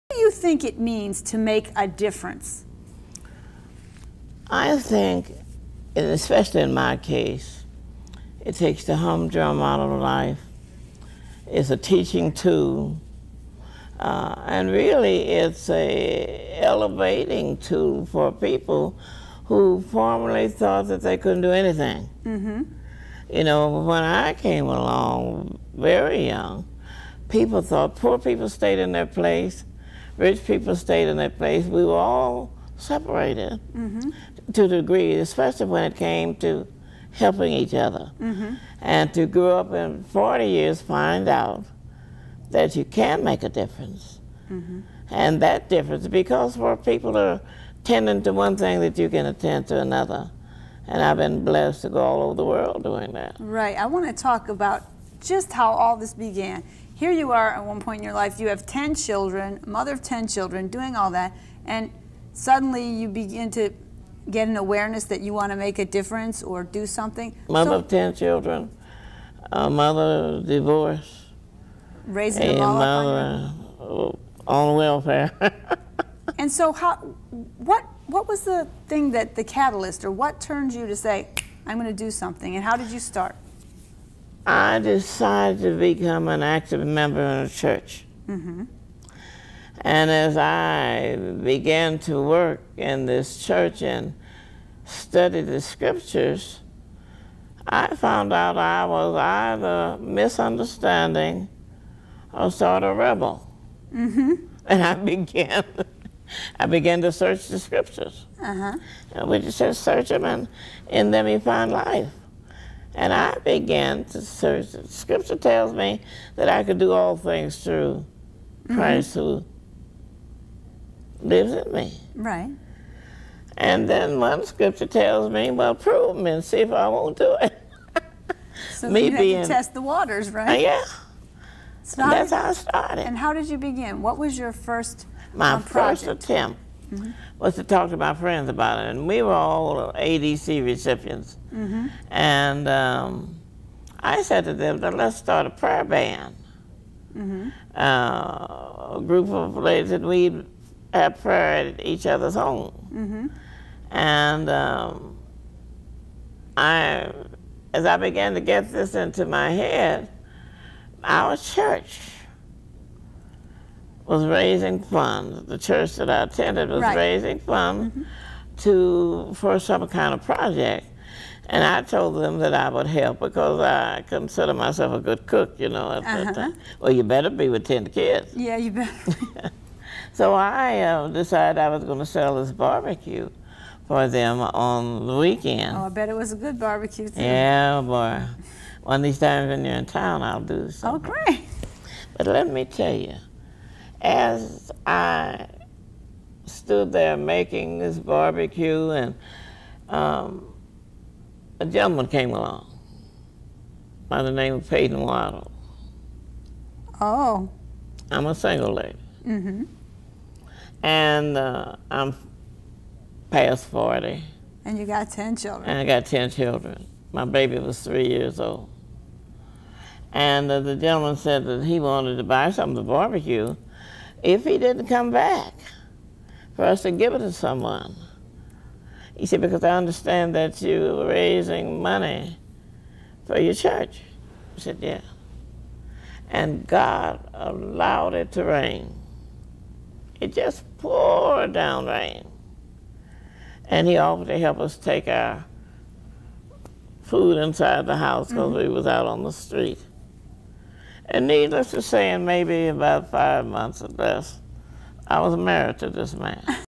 Mother Perry on what it takes to make a difference and the the BBQ that Changed her life.
On  August 23, 1996 Anne Marie  sits  down Mother Perry of the  Perpetual Mission. With an eighth-grade education in the late 1960s, Reverend Charleszetta (Mother) Waddles founded a comprehensive social services agency, the Perpetual Mission that serves the low-income communities of Detroit. James K. Davis of Life described as her "one-woman war on poverty," although she herself often struggled.

Mother" Waddles was a living saint (Detroit's Mother Teresa) without a whisper of ill will or intent